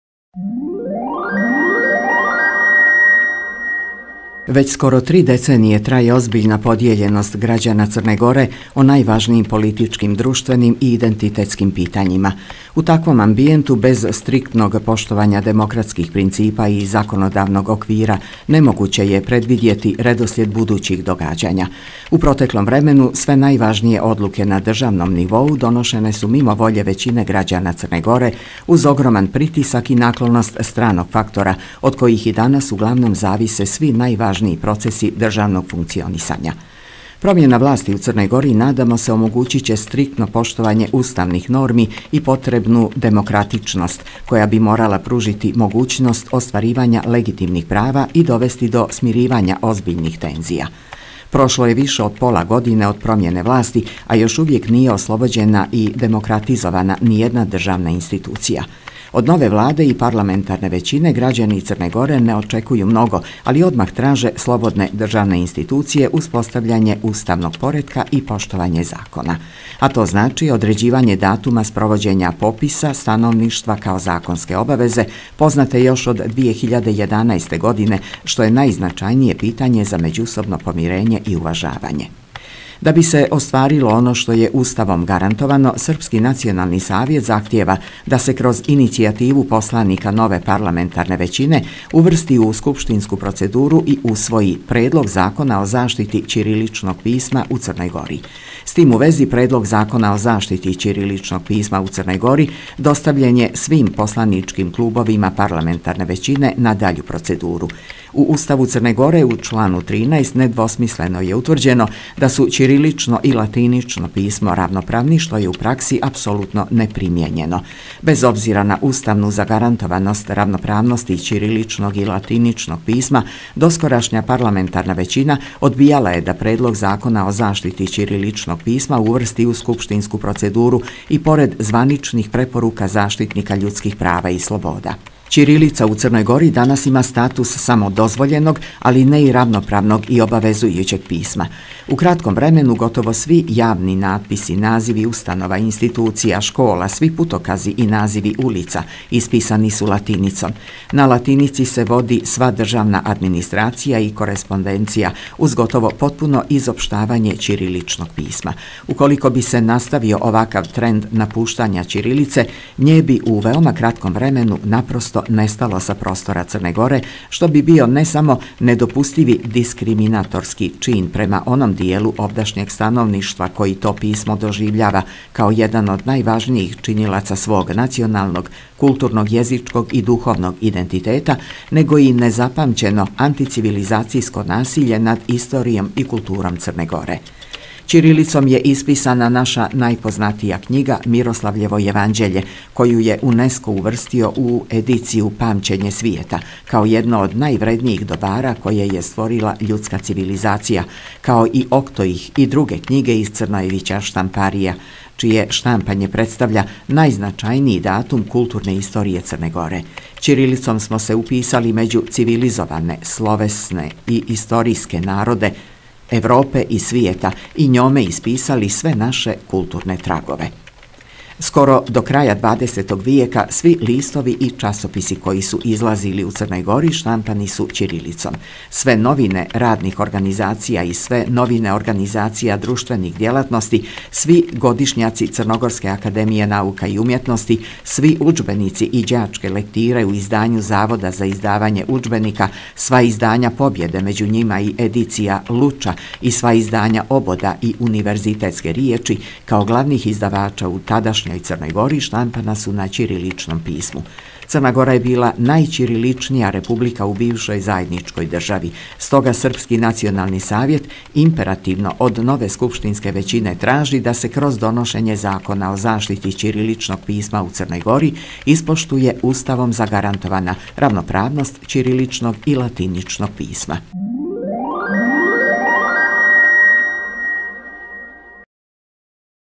Српски национални савјет: Усвојити Закон о заштити ћириличног писма у Црној Гори 22 03 2021
Већ скоро три деценије траје озбиљна подијељеност грађана Црне Горе о најважнијим политичким друштвеним и идентитетским питањима. У таквом амбијенту, без стриктног поштовања демократских принципа и законодавног оквира немогуће је предвидјети редослед будућих догађања. У протеклом времену све најважније одлуке на државном нивоу доношене су мимо воље већине грађана Црне Горе, уз огроман притисак и наклоност страног фактора од којих и данас, углавном, зависе сви најважнији процеси државног функционисања.
Промјена власти у Црној Гори, надамо се, омогућиће стриктно поштовање уставних норми и потребну демократичност, која би морала пружити могућност остваривања легитимних права и довести до смиривања озбиљних тензија. Прошло је више од пола године од промјене власти, а још увијек није ослобођена и демократизована ниједна државна институција. Од нове Владе и парламентарне већине грађани Црне Горе не очекују много, али одмах траже слободне државне институције, успостављање уставног поретка и поштовање закона. А то значи одређивање датума споровођења пописа становништва, као законске обавезе познате још од 2011. године што је најзначајније питање за међусобно помирење и уважавање.
Да би се остварило оно што је Уставом гарантовано, Српски национални савјет захтијева да се кроз иницијативу посланика нове парламентарне већине уврсти у скупштинску процедуру и усвоји предлог Закона о заштити ћириличног писма у Црној Гори. С тим у вези Предлог закона о заштити ћириличног писма у Црној Гори достављен је свим посланичким клубовима парламентарне већине на даљу процедуру.
У Уставу Црне Горе, у члану 13, недвосмислено је утврђено да су ћирилично и латинично писмо равноправни, што је у пракси апсолуно непримијењено. Без обзира на уставну загарантованост равноправности ћириличног и латиничног писма доскорашња парламентарна већина одбијала је да Предлог закона о заштити ћириличног писма уврсти у скупштинску процедуру и поред званичних препорука Заштитника људских права и слобода.
Ћирилица у Црној Гори данас има статус само дозвољеног, али не и равноправног и обавезујућег писма. У кратком времену, готово сви јавни натписи, називи установа, институција, школа, сви путокази и називи улица, исписани су латиницом. На латиници се води сва државна администрација и коресподенција, уз готово потпуно изопшавање ћириличног писма. Уколико би се наставио овакав тренд напуштања ћирилице, ње би у веома кратком времену напросто нестало са простора Црне Горе, што би био не само недопустиви дискриминаторски чин према оном дијелу овдашњег становништва који то писмо доживљава као један од најважнијих чинилаца свог националног, културног, језичког и духовног идентитета, него и незапамћено антицивилизацијско насиље над историјом и културом Црне Горе.
Ћирилицом је исписана наша најпознатија књига „Мирослављево јеванђеље“, коју је Унеско уврстио у едицију „Памћење свијета“, као једно од највреднијих добара које је створила људска цивилизација, као и „Октоих“ и друге књиге из Црнојевића штампарија, чије штампање представља најзначајнији датум културне историје Црне Горе. Ћирилицом смо се уписали међу цивилизоване (словесне) и историјске народе Европе и свијета и њоме исписали све наше културне трагове.
Скоро до краја 20. вијека сви листови и часописи који су излазили у Црној Гори штампани су ћирилицом. Све новине радних организација и све новине организација друштвених дјелатности, сви годишњаци ЦАНУ, сви уџбеници и ђачке лектире у издању Завода за издавање уџбеника, сва издања „Побједе“, међу њима и едиција „Луча“ и сва издања „Обода“ и „Универзитетске ријечи“, као главних идавача у тадашњој Црној Гори, штампана су на ћириличном писму. Црна Гора је била најћириличнија република у бившој заједничкој држави. Стога Српски национални савјет императивно од нове скупштинске већине тражи да се кроз доношење Закона о заштити ћириличног писма у Црној Гори испоштује уставом загарантована равноправност ћириличног и латиничног писма.
У Подгорици, 22. марта 2021. године
ПРЕДСЈЕДНИК
Др Момчило Вуксановић